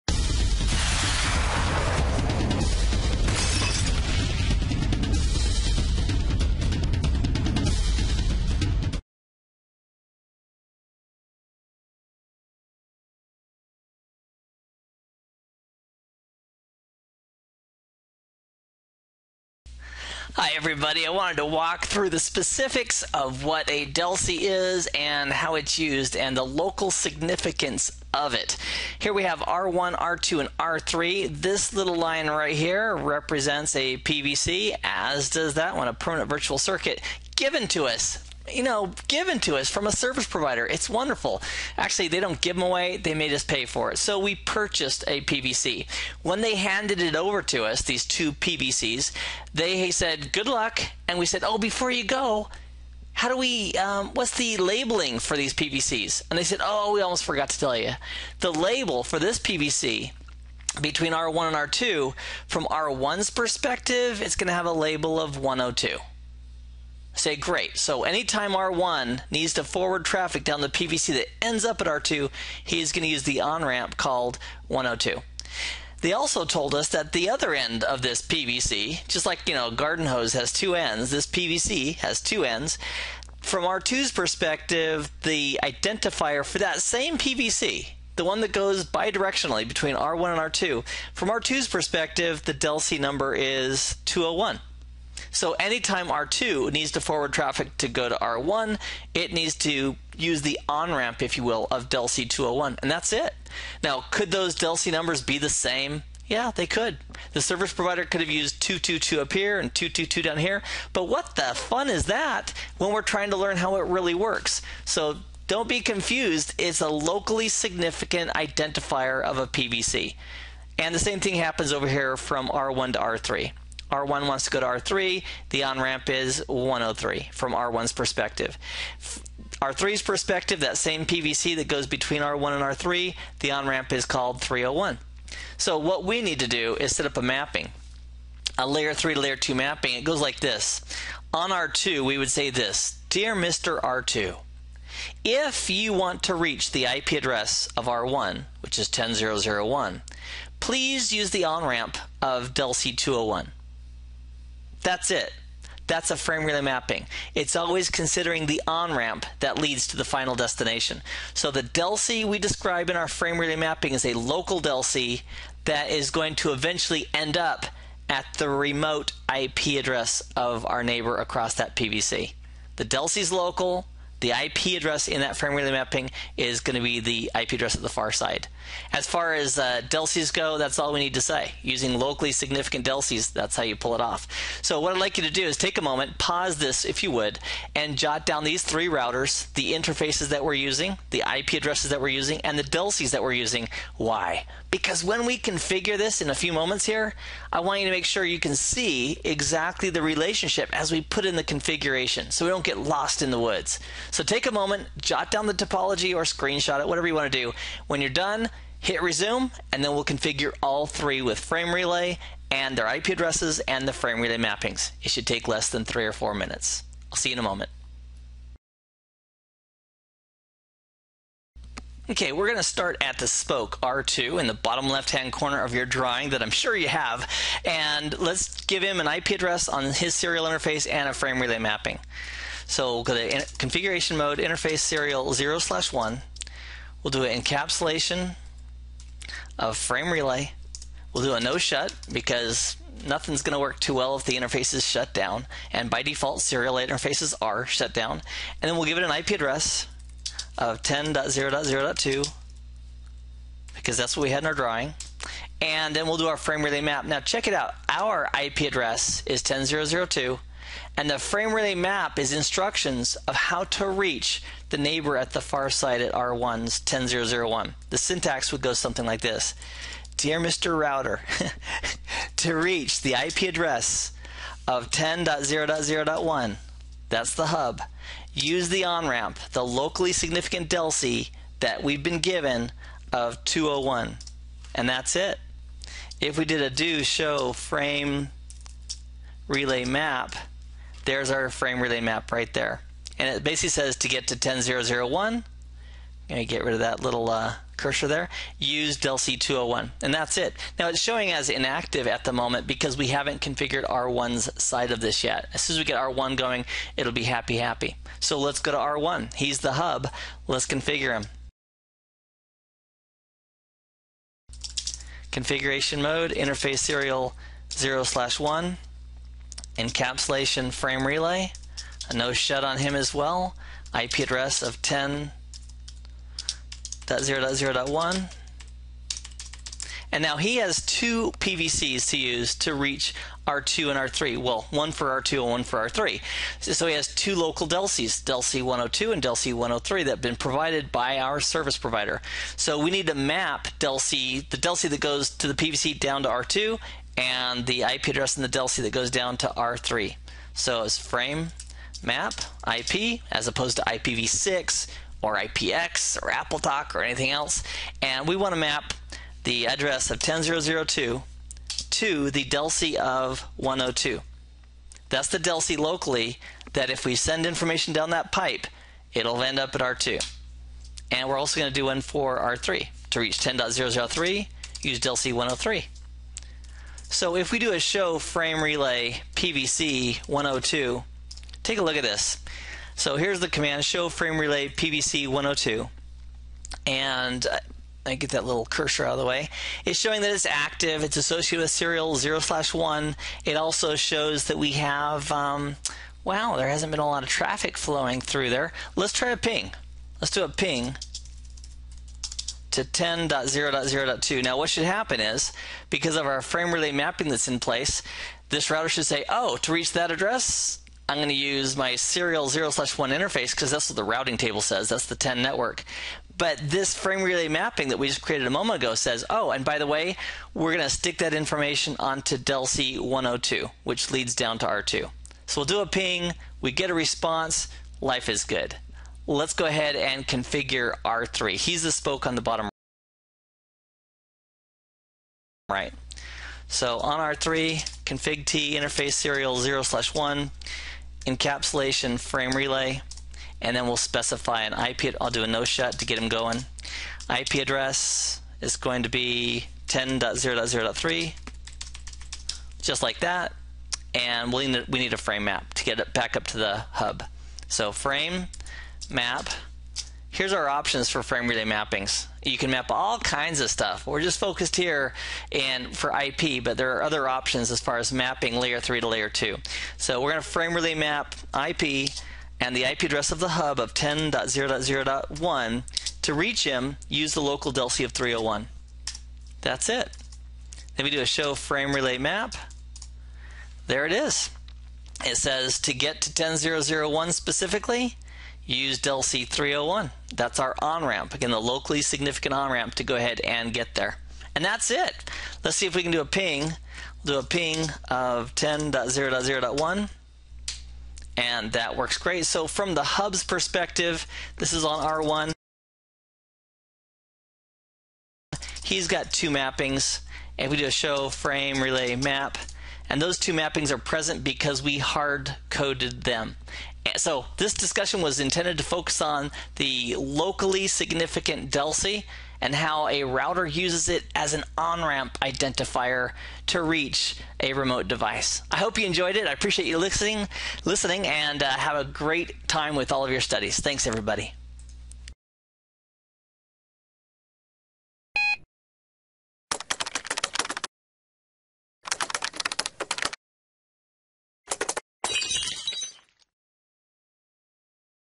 Frame Relay DLCI mappings
You have questions, this video has the answers regarding the Data Link Connection Identifier (DLCI).